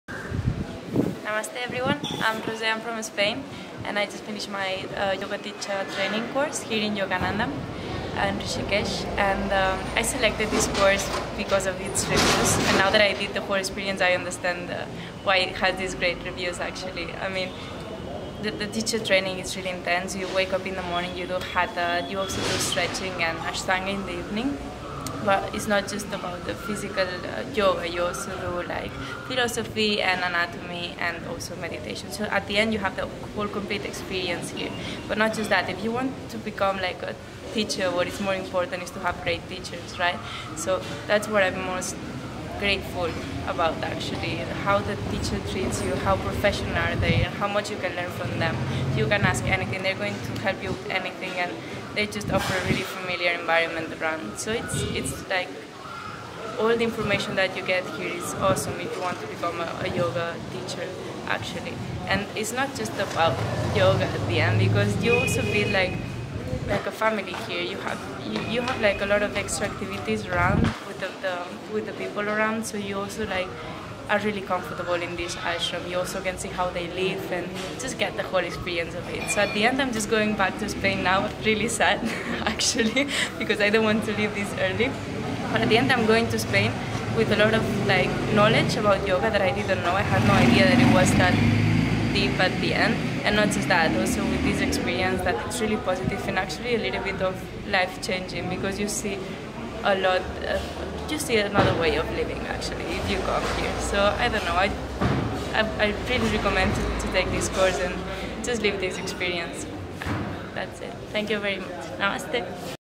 200 hour Yoga Teacher Training student Reveiw about Yoganandham Yoga School
Our student Roser from Spain has completed her Yoga Teacher Training course at Yoganandham Rishikesh. A very big thanks from Yoganandham Team for this beautiful about our Yoga School